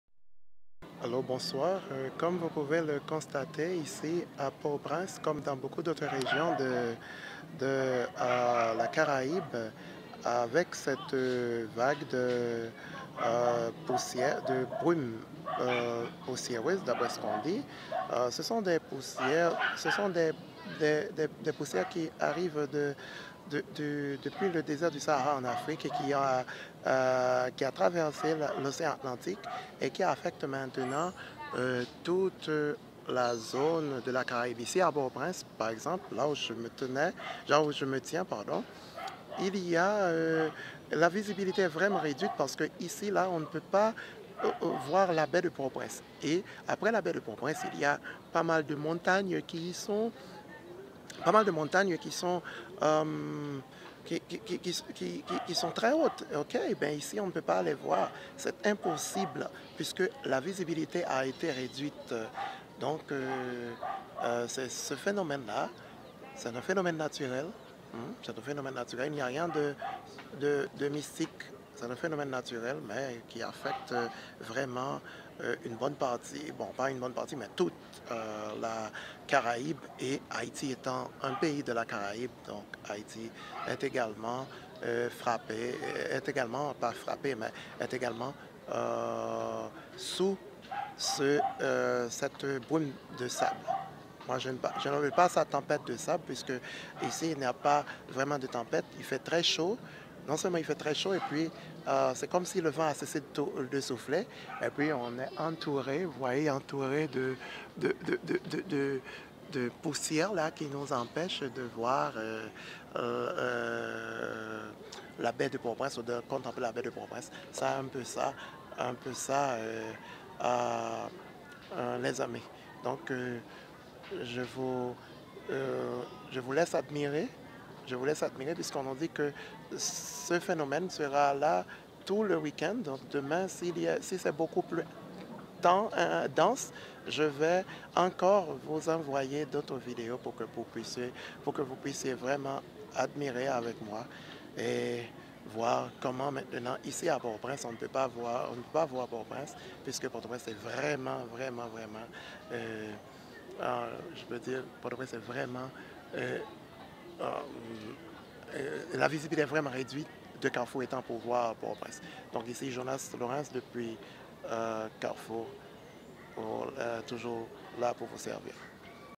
La brume de sable reduit la visibilite sur toute Haiti, ici c'est la Baie de Port-au-Prince
C'est sans conteste la brume de sable la plus dense que j'aie observé sur Haïti depuis au moins 30 ans. On ne voit plus les montagnes au nord de Port-au-Prince.
Rudy Mr Meteo sur sa page Facebook